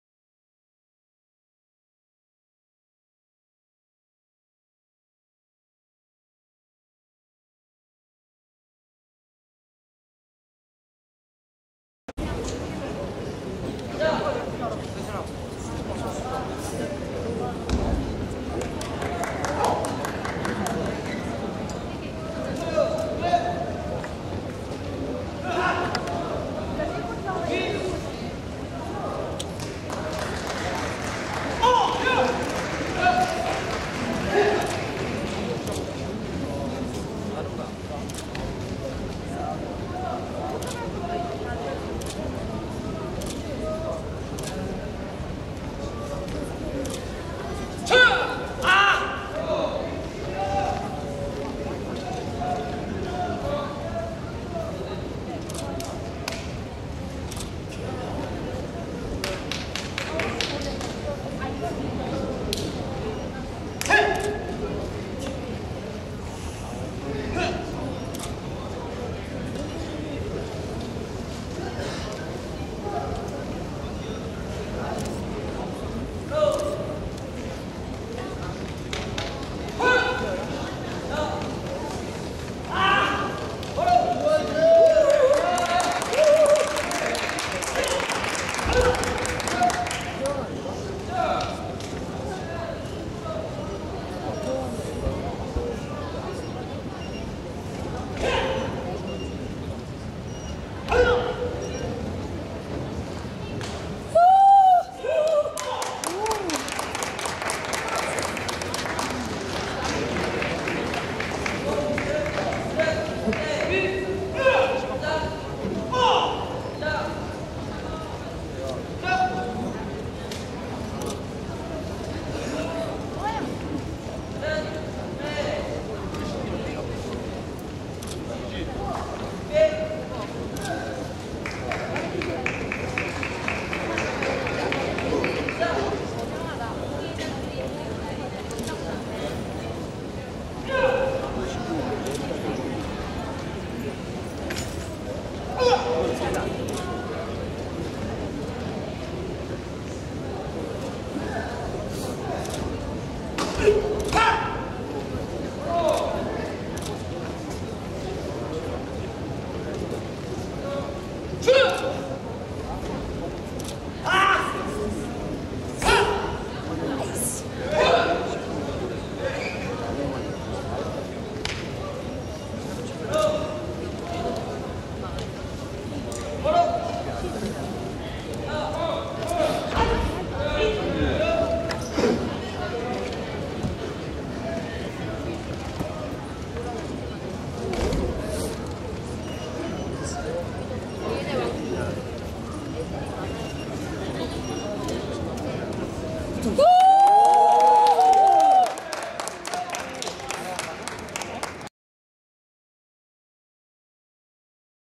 제10회우석대총장기 남자장년2부 김진(독수리)vs우하성(선인)준결승
제10회우석대총장기 남자장년2부 김진(독수리태권도장)vs우하성(선인태권도장)준결승
Taekwondo_poomsae
2012년 7월13일 ~ 15일 / 3일간